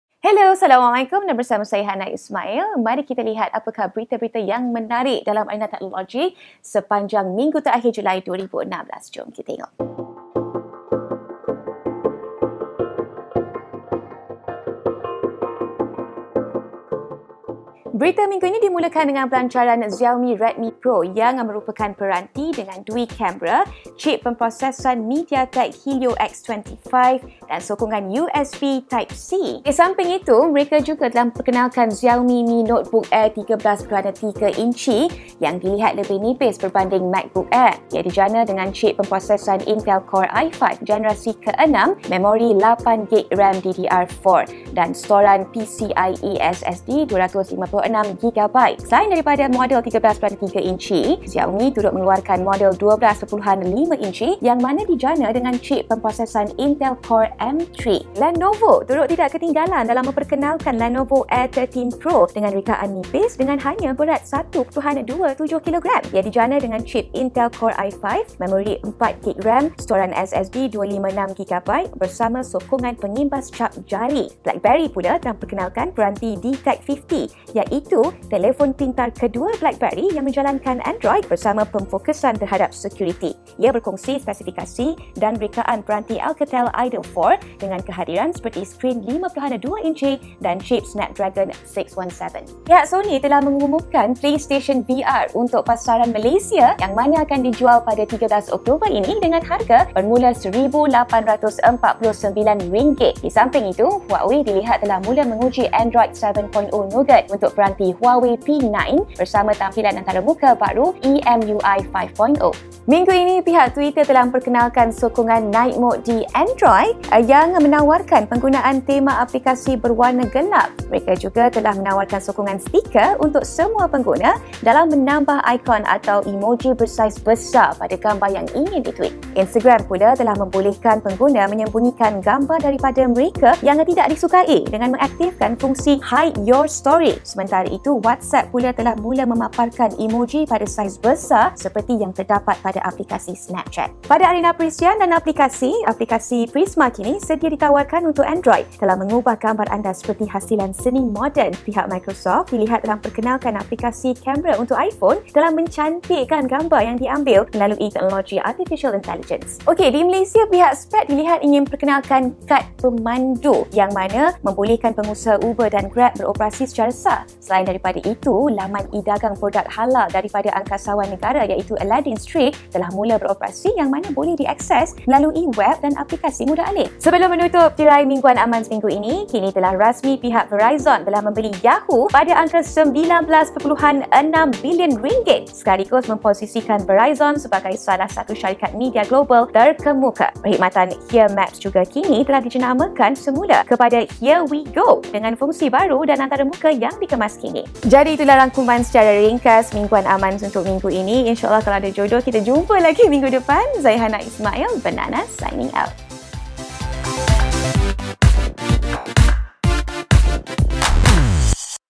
Mingguan Amanz - Xiaomi Mi Notebook, Prisma, PlayStation VR Malaysia
Xiaomi pada minggu ini telah memperkenalkan dua peranti baru, iaitu sebuah telefon pintar yang dinamakan sebagai Redmi Pro, dan satu lagi adalah komputer riba berasaskan Windows 10 - dinamakan sebagai Xiaomi Mi Notebook Air.

BlackBerry turut mencuri perhatian dengan mengumumkan telefon Android kedua, tetapi kali ini membawakan skrin sentuh sepenuhya.

Sony pada minggu ini telah mengumumkan PlayStation VR untuk pasaran tempatan. Peranti realiti-maya ini akan dijual bermula 13 Oktober 2016 ini pada harga RM1849.

Dalam arena aplikasi pula, Twitter telah memperkenalkan sokongan Night Mode untuk pengguna Android. Satu lagi aplikasi popular yang telah hadir ke peranti Android minggu ini adalah Prisma - yang membolehkan anda mengubah gambar kepada bentuk seni moden.